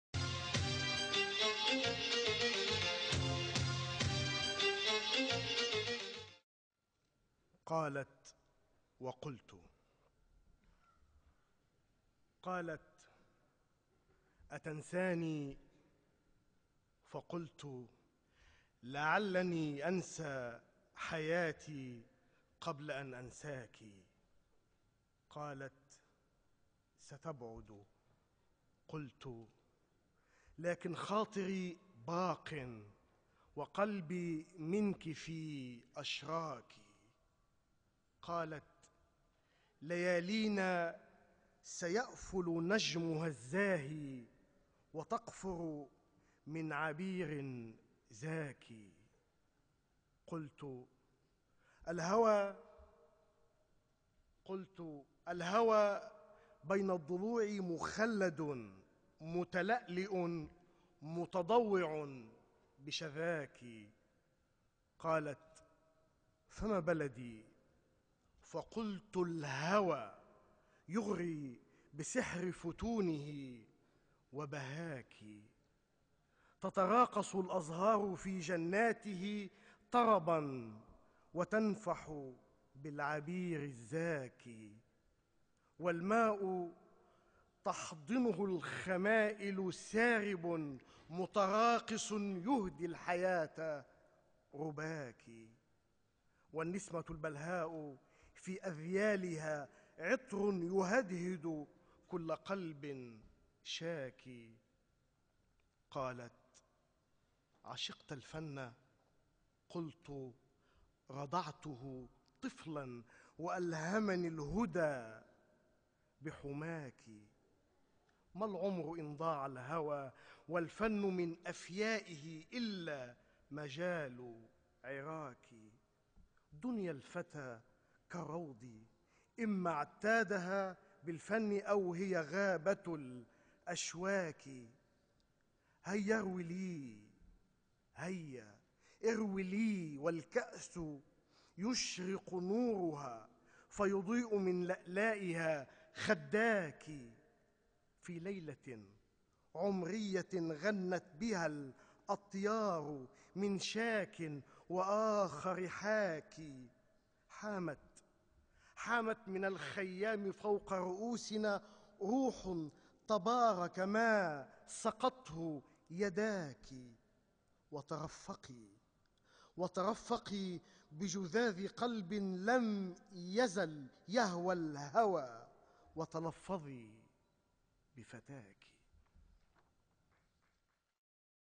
الشاعر الشيخ صقر بن سلطان القاسمي قصيدة " قالت وقلتُ "
الشاعر الشيخ صقر بن سلطان القاسمي .. من الأعمال الشعرية الكاملة له .. قصيدة " قالت وقلتُ " إلقاء الشاعر أحمد الشهاوي